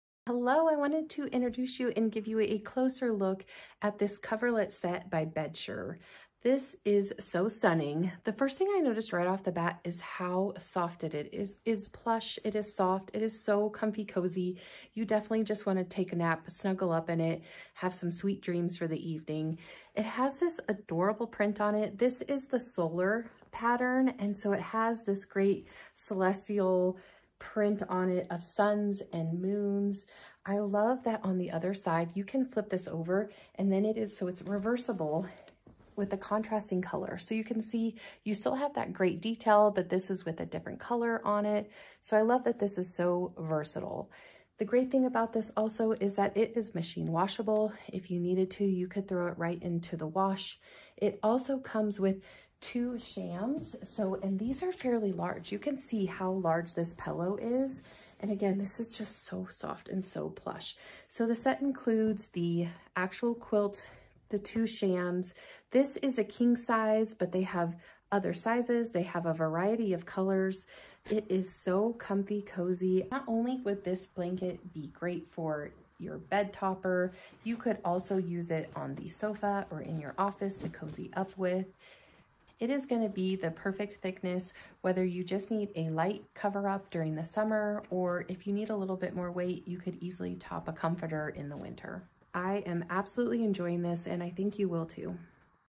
Bedsure Solar Pattern Coverlet - Lightweight Summer Quilt Set
Sophisticated Style: This quilt set features a quilted pattern that combines the sun and stars, bringing a touch of elegance to your living space.
Softness That Lasts: This quilt set is prewashed for an exceptional soft hand feel, and it gets even softer after washes. Its anti-static and breathable fabric provides maximum comfort without any crinkly noise.
Versatile and Practical: This bedspread is versatilely practical for various use cases, including as a thin summer quilt, as an extra lightweight bedspread in the winter, as a bed coverlet for pet, or as a simple and elegant bedding decoration element.
Easy Care: This bedspread coverlet is machine-washable and easy to maintain. Simply machine wash in cold water on gentle cycles and tumble dry low for a like-new restoration.
What You Can Get: This Queen Quilt Set comes with a quilt (90" x 96") and a matching pillow sham (20" x 26" + 2").